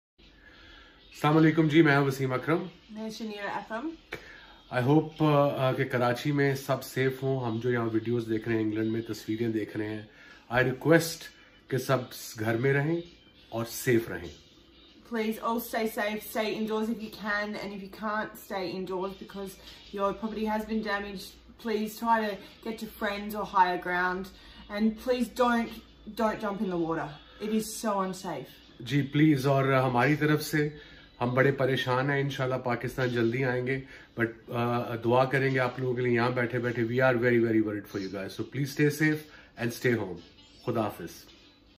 Wasim Akram and wife Shaniera Akram request the people of Karachi to stay home amidst heavy rain
Former Pakistani cricketer Wasim Akram and his wife Shaniera Akram request the people of Karachi to stay home amidst heavy rain